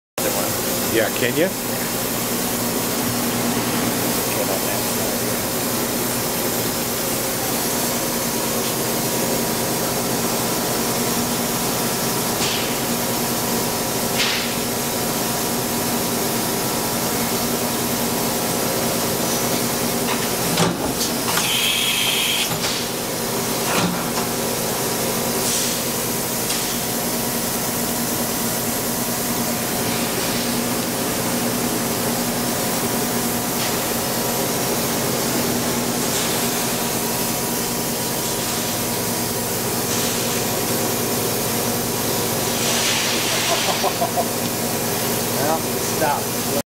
Video Leblond VMC Model FNC 60
Leblond Makino Vertical Machining Center
Model FNC 60
 Fanuc O-M Control